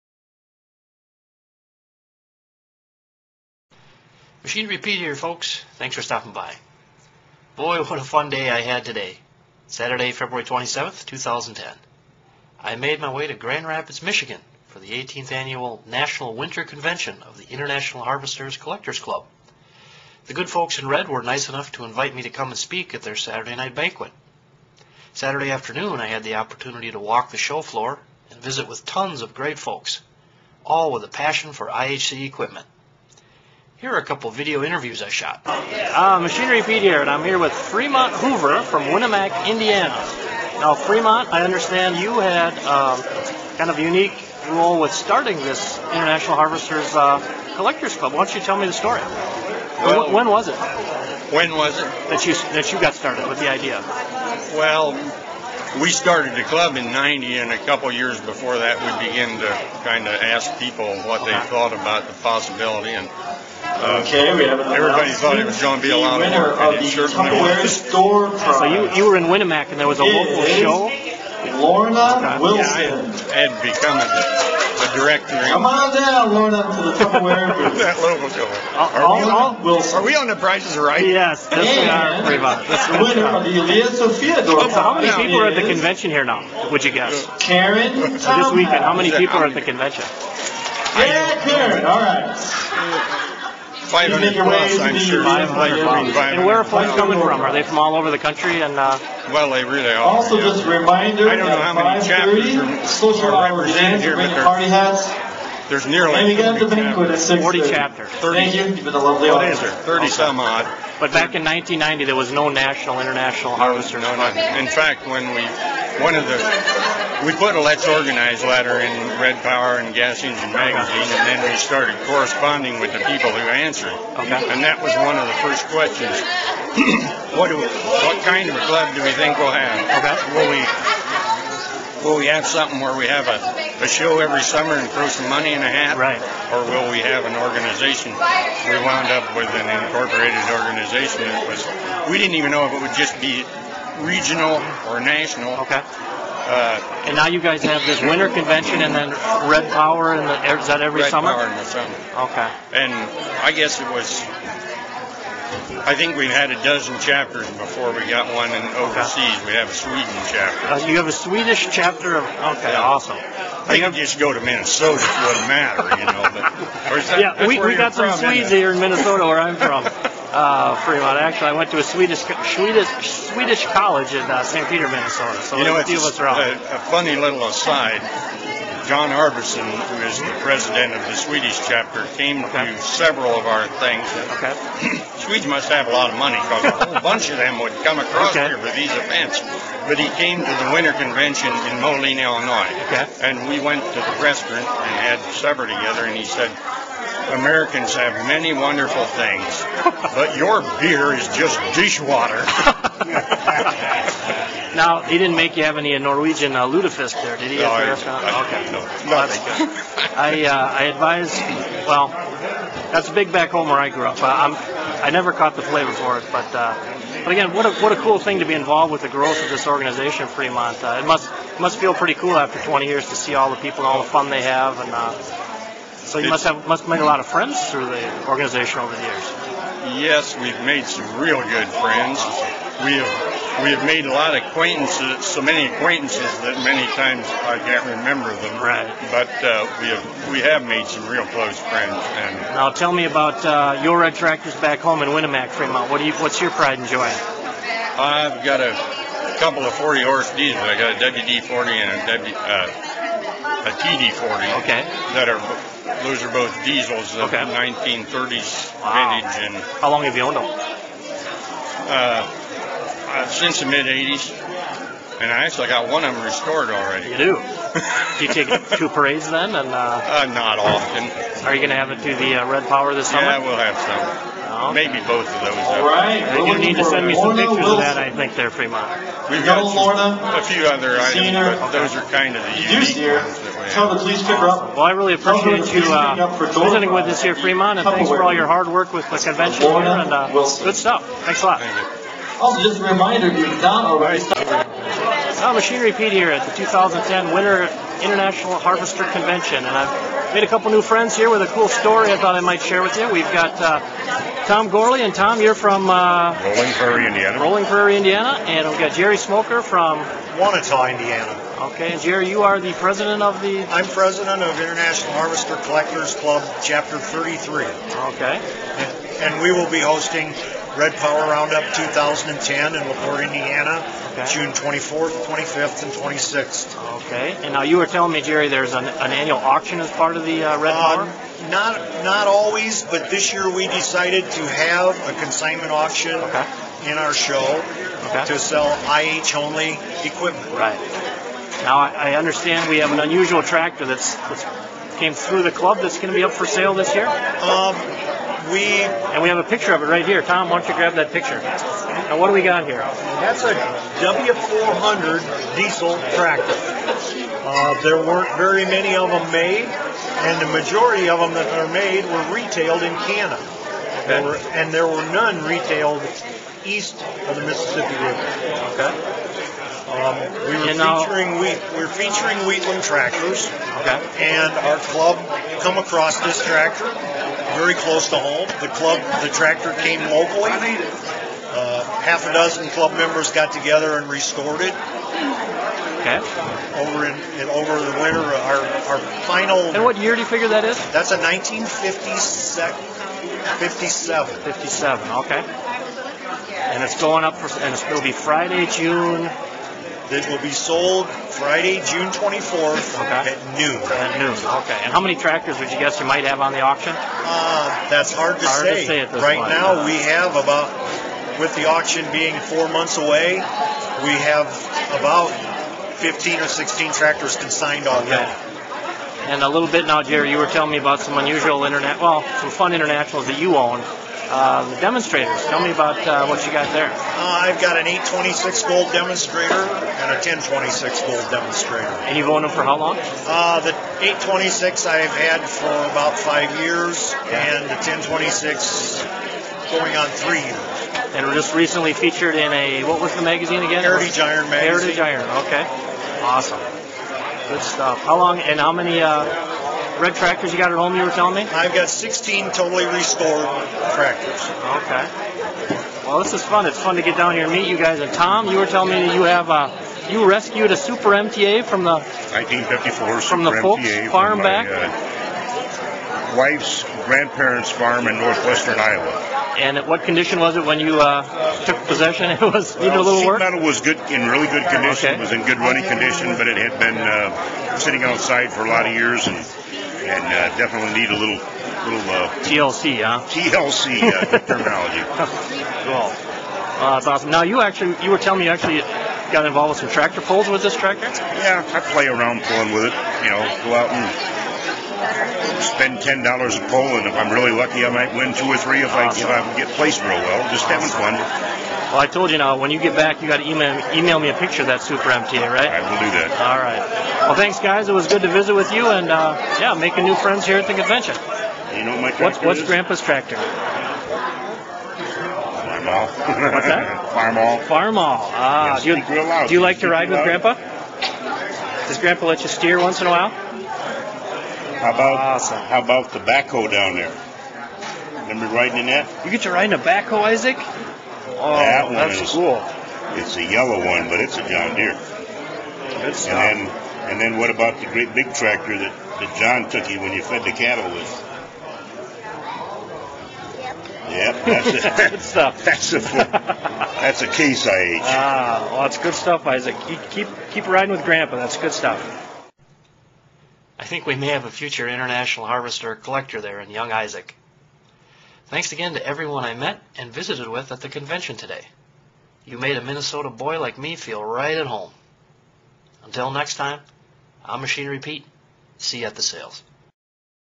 IHC Collectors Convention: February 27, 2010
Machinery Pete drops in to the 18th Annual National Winter Convention of the IHC Collectors Club in Grand Rapids, MI. Pete interviews Fremont Hoover and Jerry Smoker, both members of the northern Indiana chapter of the IHC Collectors Club. Hoover was instrumental in helping start the IHC Collectors Club back 20 years ago.